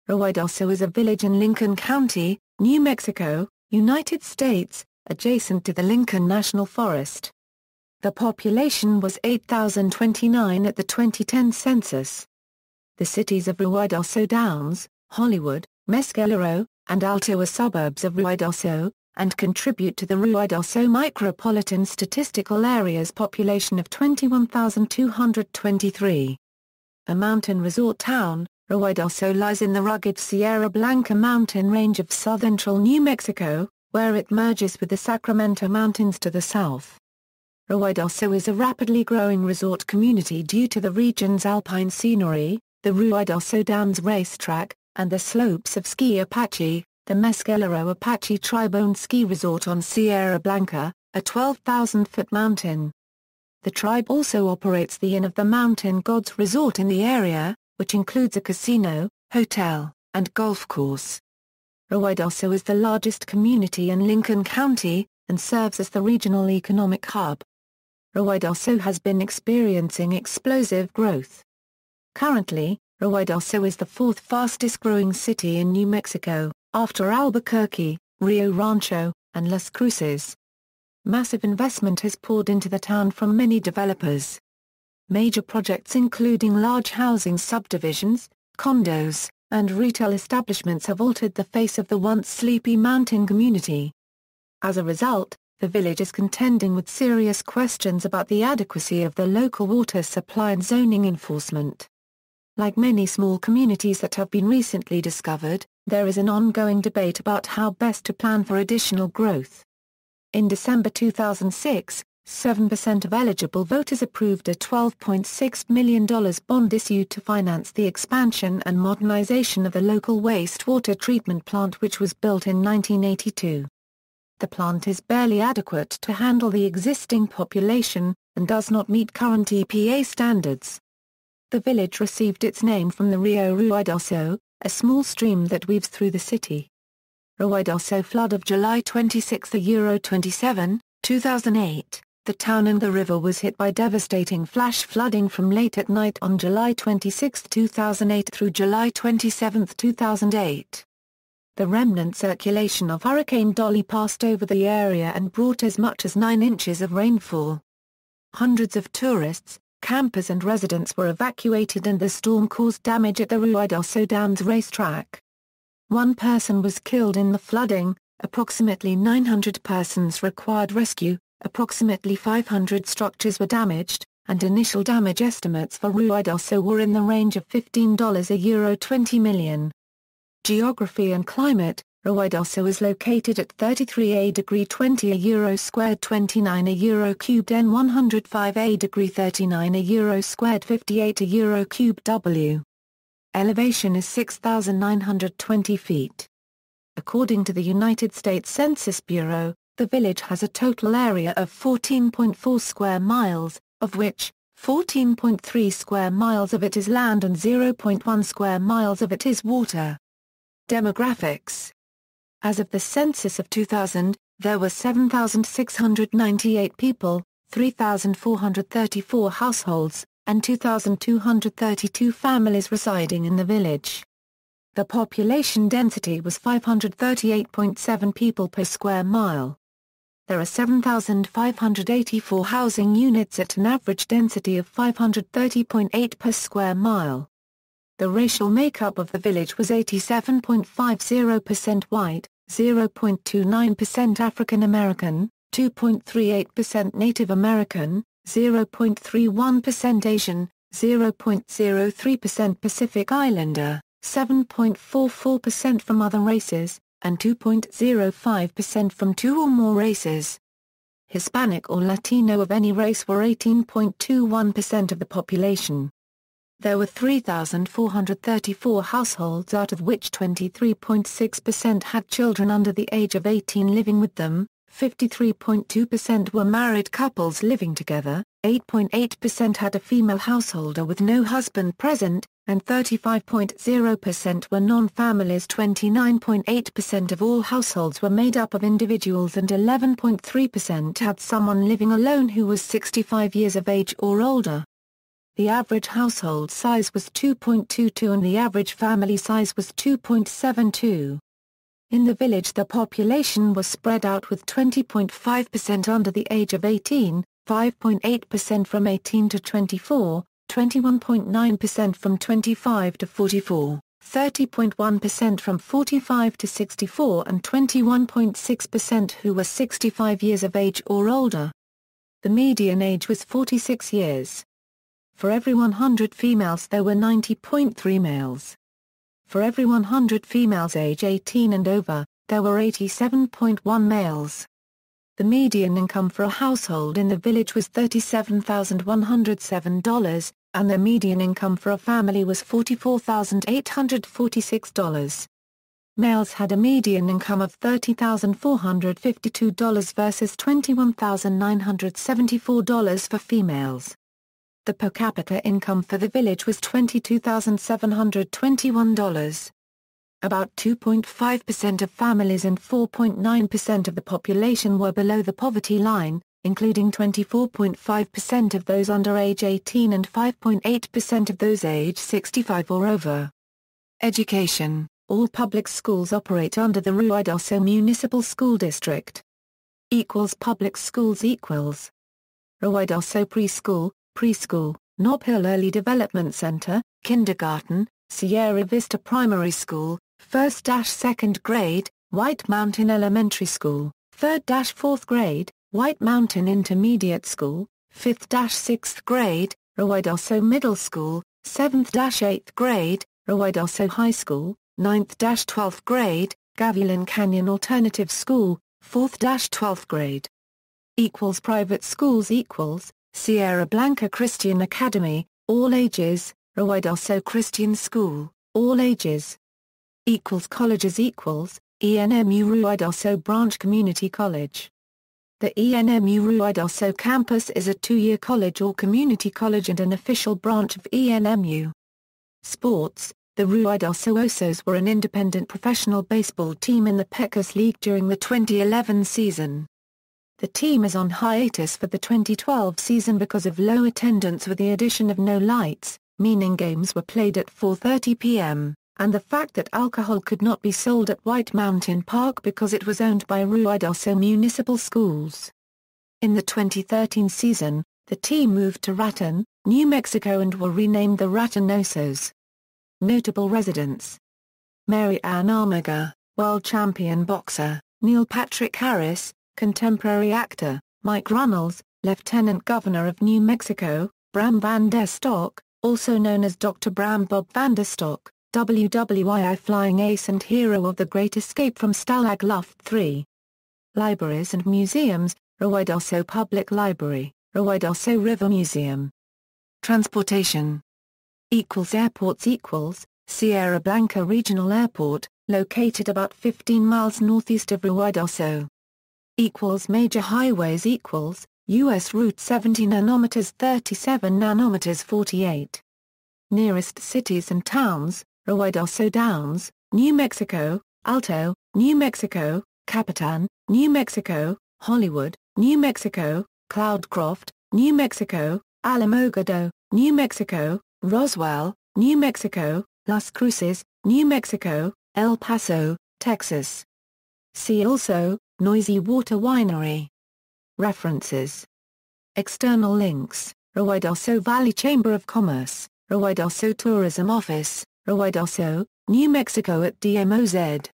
Ruidoso, New Mexico
Ruidoso is a village in Lincoln County, New Mexico, United States, adjacent to the Lincoln National Forest. The population was 8,029 at the 2010 census. The cities of Ruidoso Downs, Hollywood, Mescalero, and Alto are suburbs of Ruidoso, and contribute to the Ruidoso Micropolitan Statistical Area's population of 21,223.
A mountain resort town, Ruidoso lies in the rugged Sierra Blanca mountain range of southcentral New Mexico, where it merges with the Sacramento Mountains to the south. Ruidoso is a rapidly growing resort community due to the region's alpine scenery, the Ruidoso Downs racetrack, and the slopes of Ski Apache, the Mescalero Apache Tribe-owned ski resort on Sierra Blanca, a 12,000-foot mountain. The tribe also operates the Inn of the Mountain Gods resort in the area, which includes a casino, hotel, and golf course. Ruidoso is the largest community in Lincoln County, and serves as the regional economic hub.